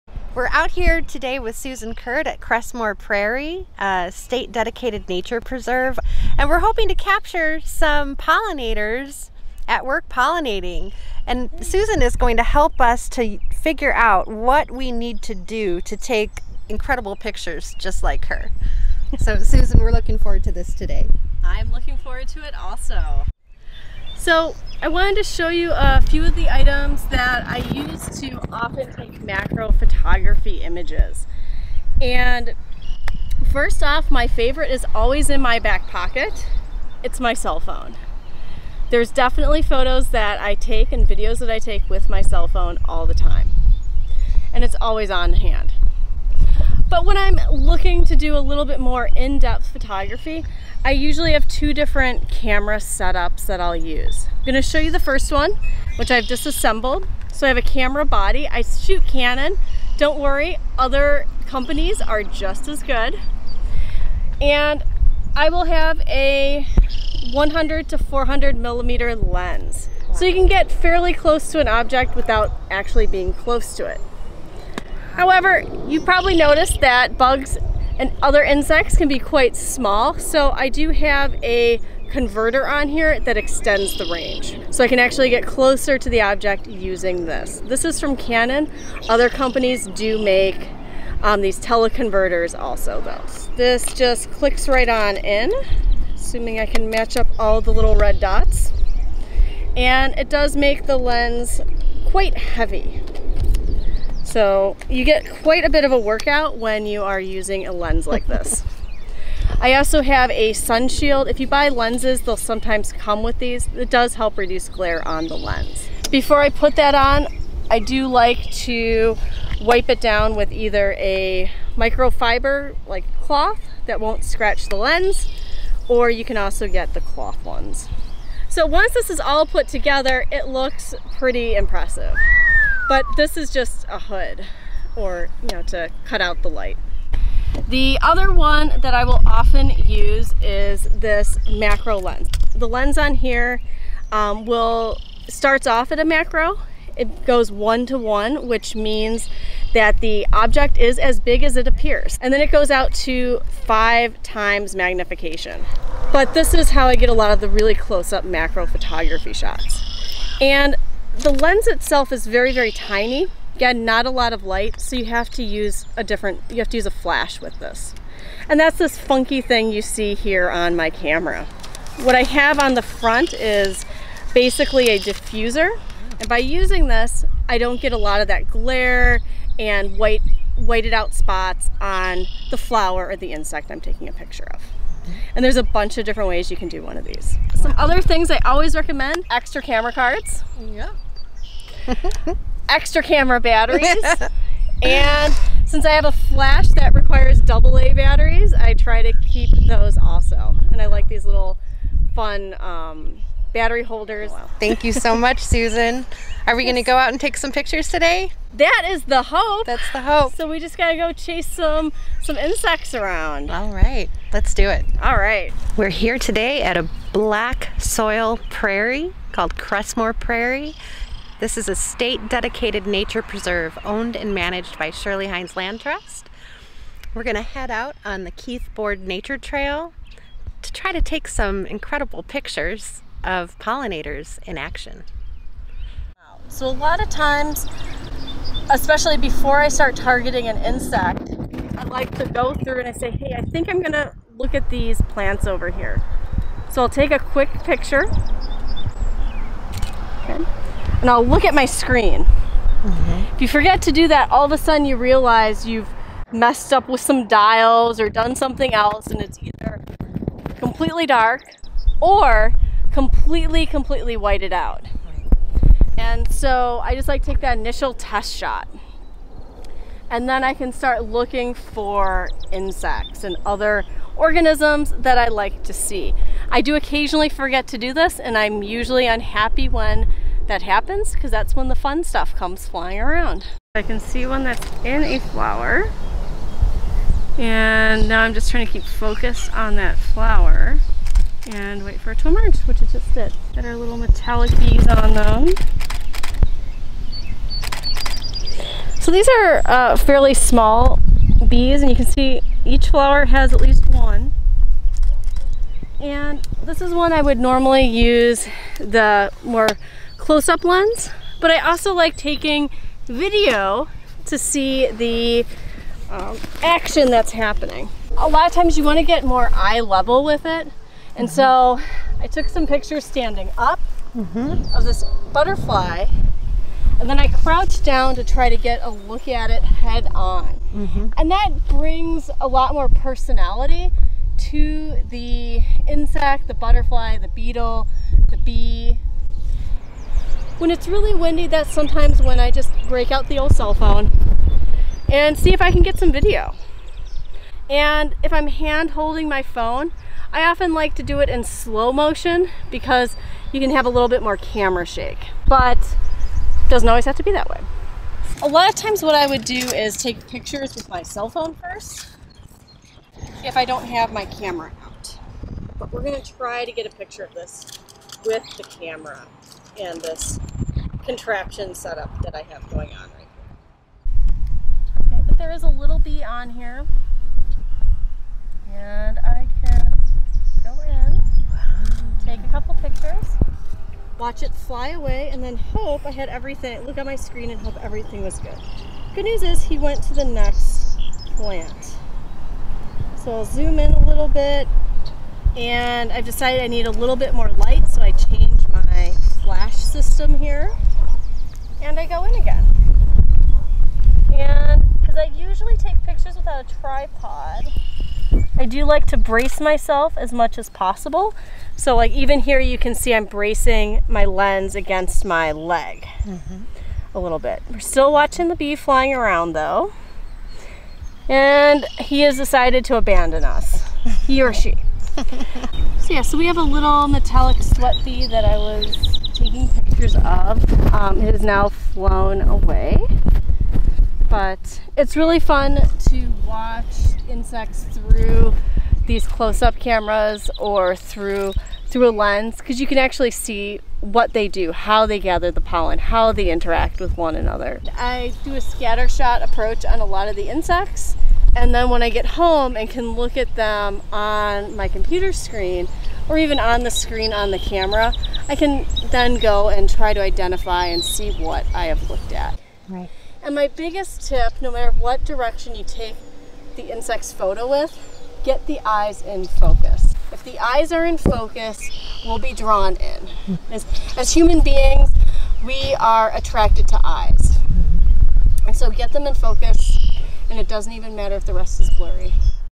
Photographing Pollinators
Explore the unseen world around you with Susan Kirt of Susan Kirt Photography through macro photography, capturing close-up images of pollinators in action in our gardens and local preserves.